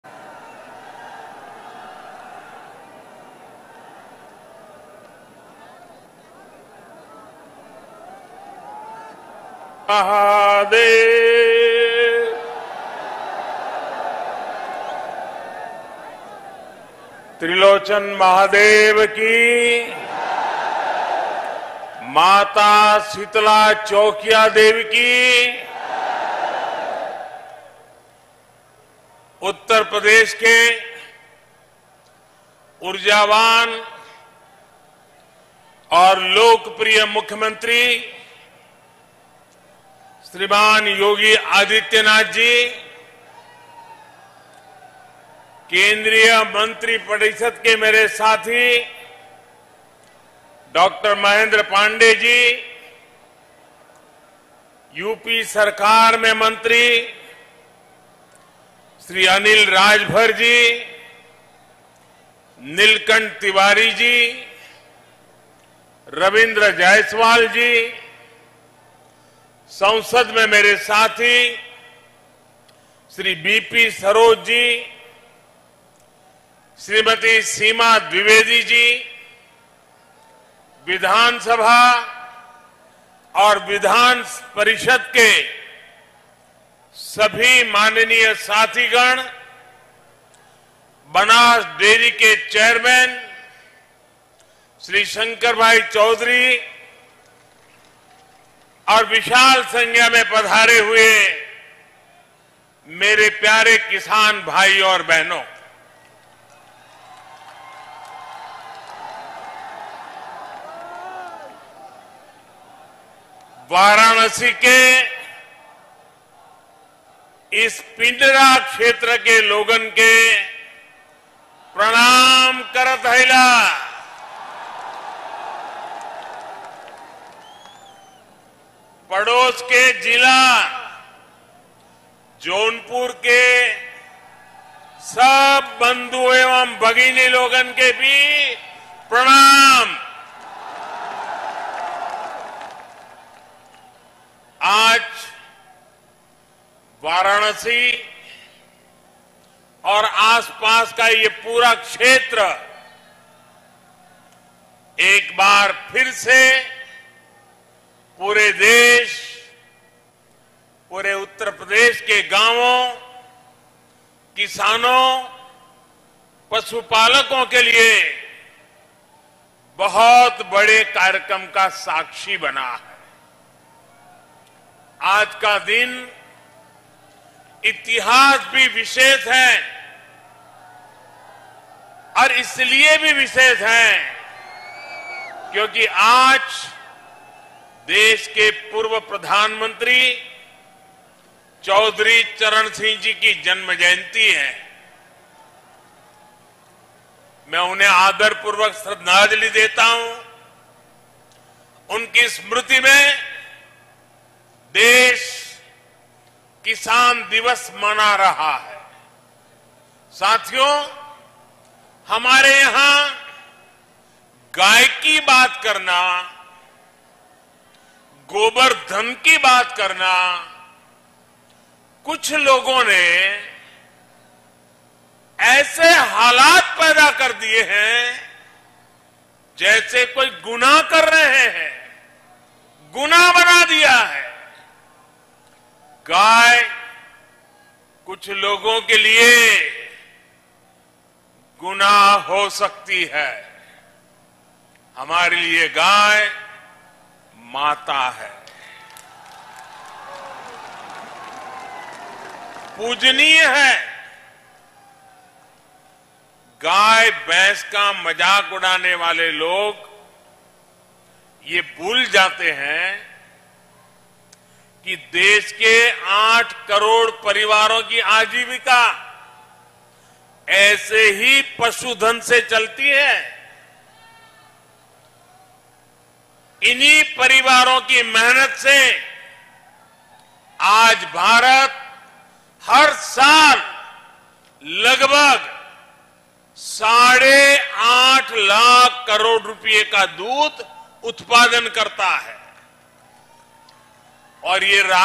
Varanasi से Narendra Modi ने लगाया हर हर महादेव का नारा और फिर जनता ने जो किया
VK News Live! Hindi News| Breaking News | Latest News